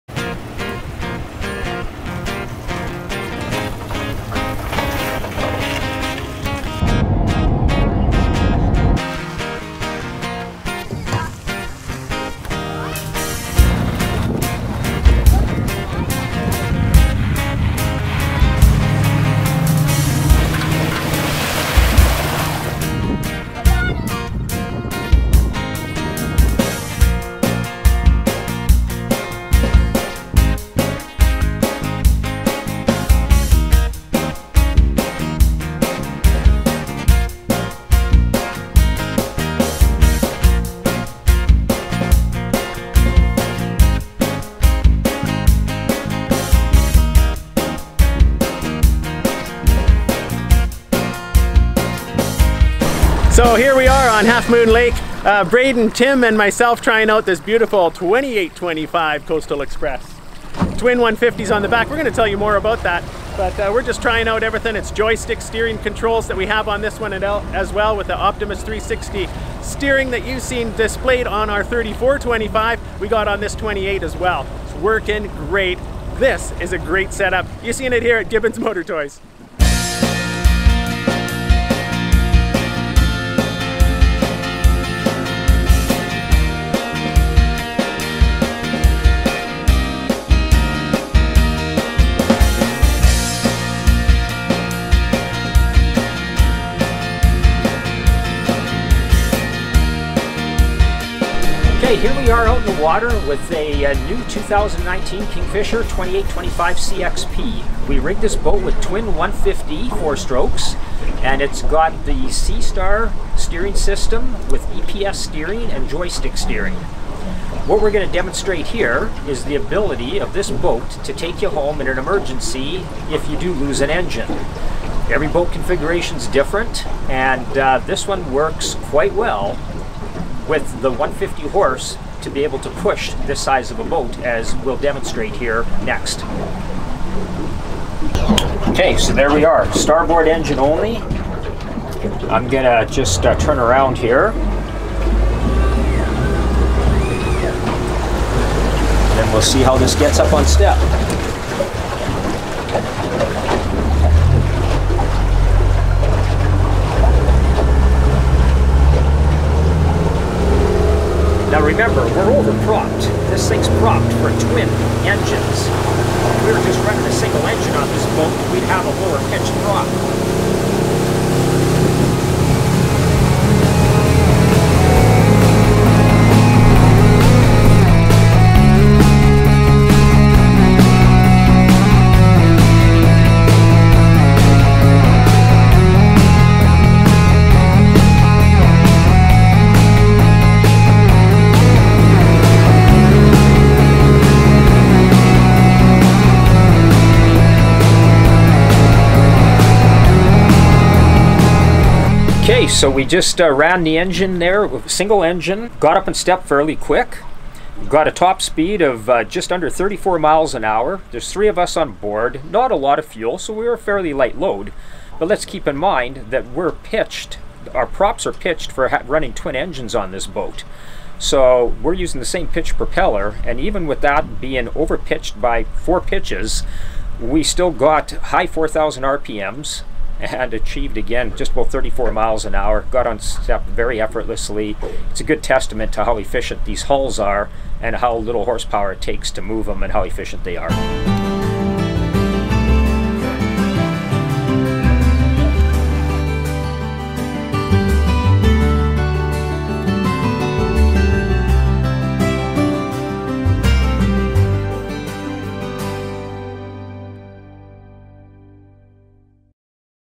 kingfisher 2825 coastal w twins new
Can your twin engine boat achieve plane with just one engine? We test this Kingfisher 2825 Coastal Express with twin 150hp 4 stroke Mercs.
Spencer at Gibbons Motor Toys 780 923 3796